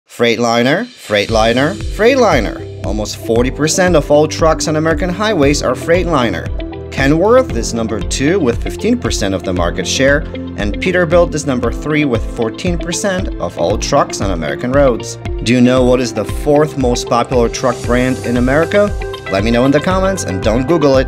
Can you guess the #4?
These are the three most popular class 8 semi trucks in America. Do you know what #4 is?